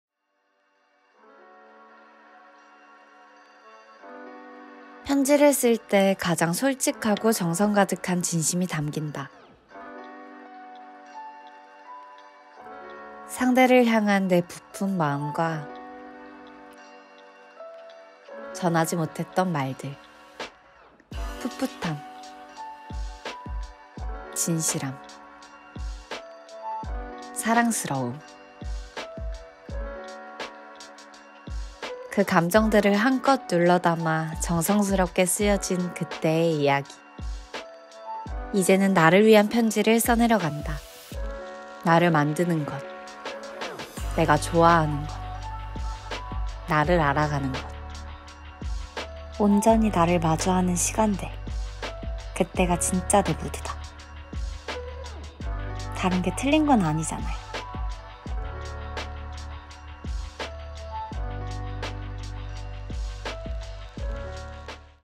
MOOD_ PRESENTARSE [문성주]
'무드' 라는 단어를 배우만의 생각으로 표현한 작품입니다.